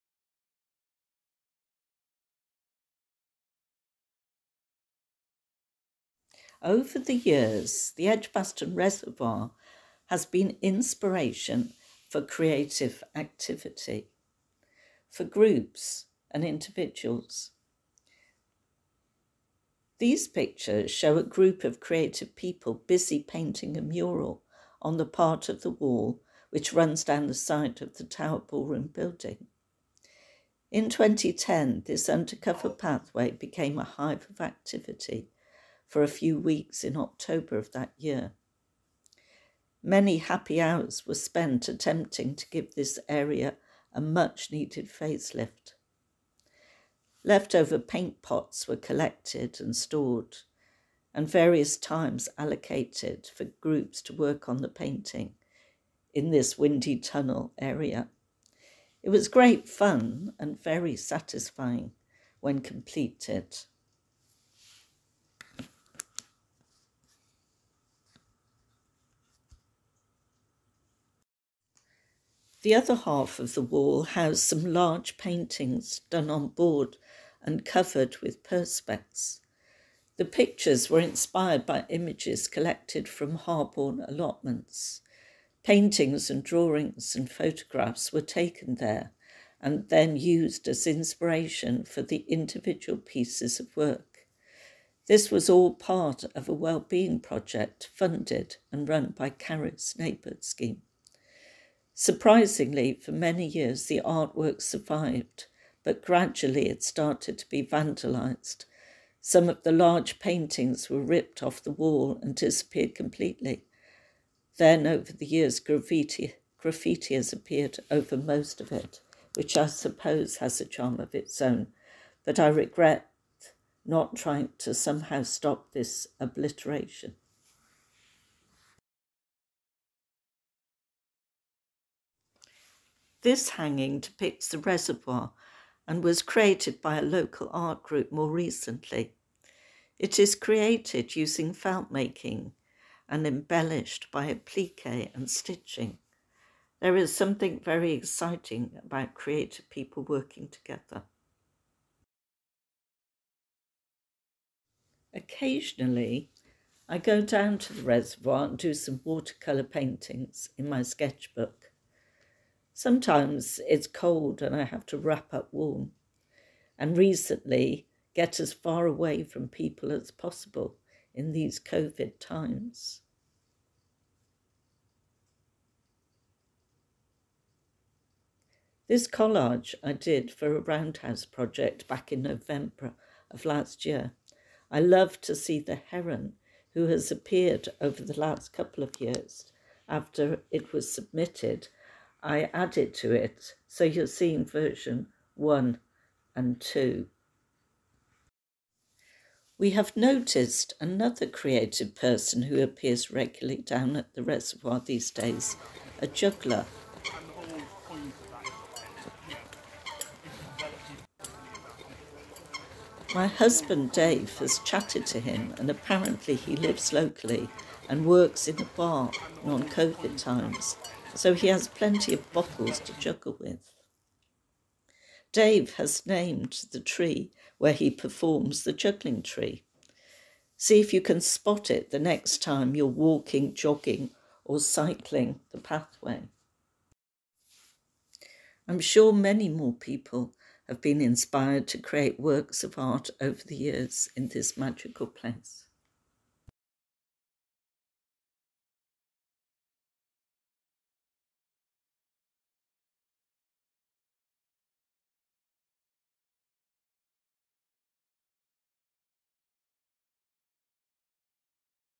The Creative Reservoir
Video by Elizabeth Naylor about Creativity by the Edgbaston Reservoir
This video is part of the " My Reservoir" Project, telling stories of the Edgbaston Reservoir in Birmingham , UK